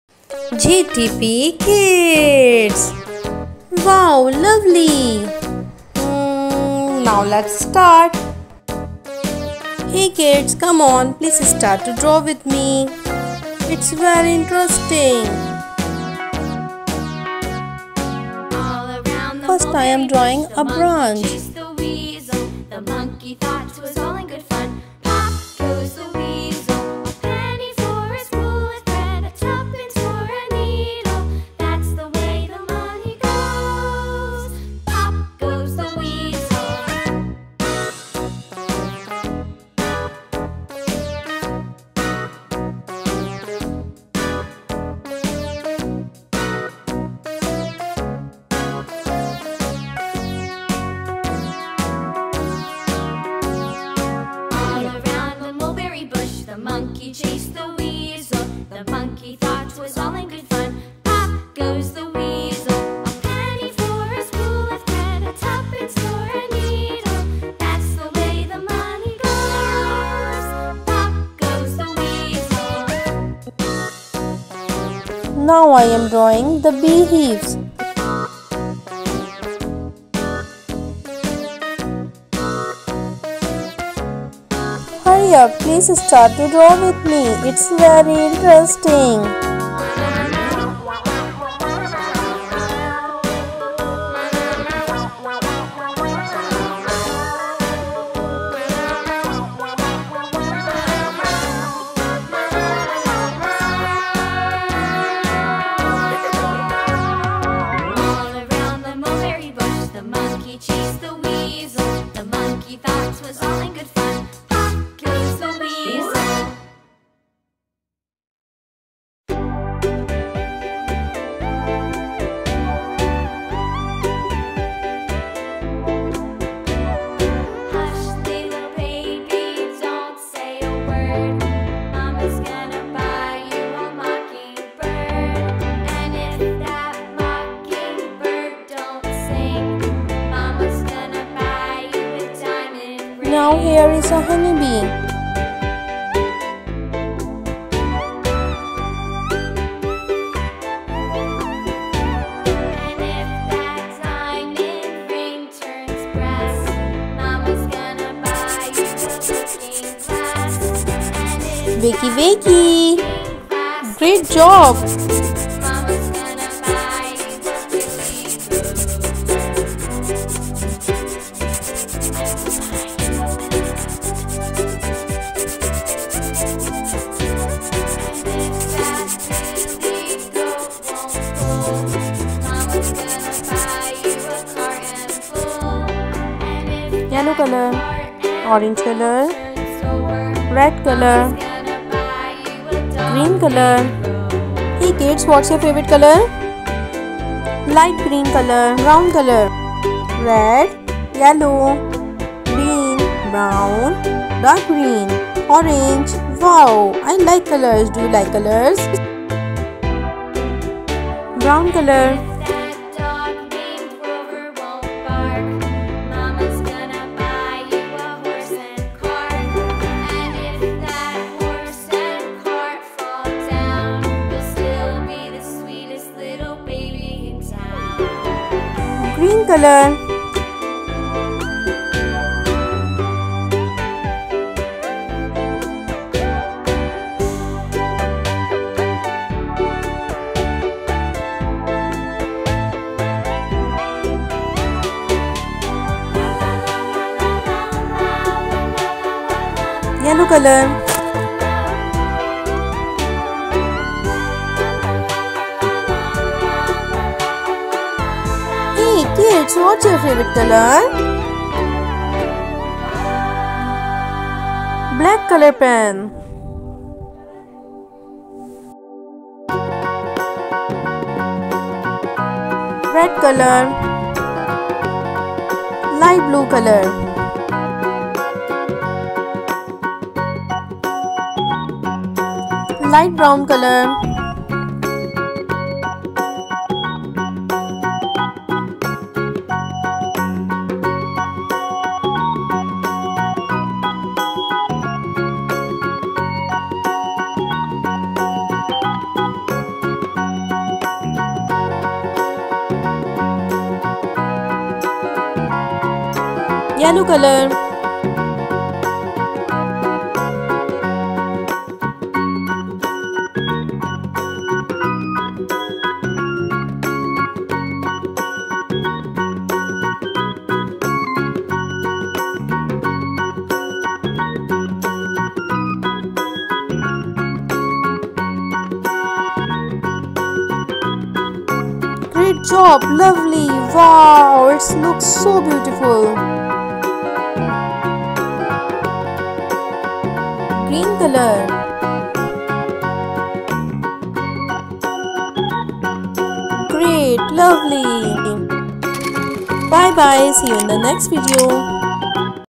Honeybee and Bee Hives Drawing Coloring for Kids and Toddlers
Hey Kids Today we a gonna Draw Honey Bee and Bee Hives Drawing,Coloring for Kids and Toddlers
hey Kids today we a gonna draw Colorful House please start to draw with me it's very interesting

I love ❤ drawing Art and sharing with super cool kids and Toddlers